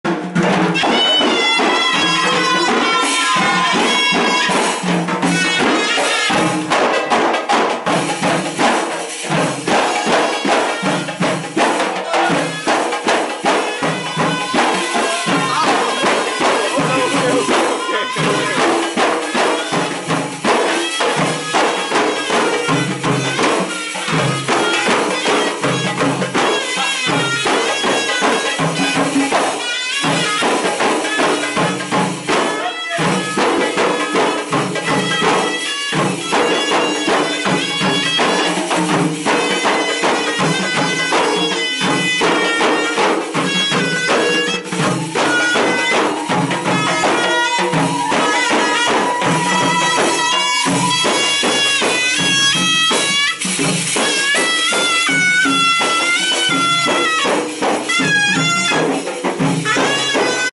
Egyptian Wedding Procession - Modern (HD)
Just my luck, as i was sitting down in the hotel lobby, on my first night ever in Egypt, I hear loud music and drums! And as my luck would have it, a wedding procession was on the way through the lobby, I asked if they mind if i take a video, they didn't mind at all! As a matter of fact they invited me! But of course i had to politely decline and let them enjoy their special day with family and friends.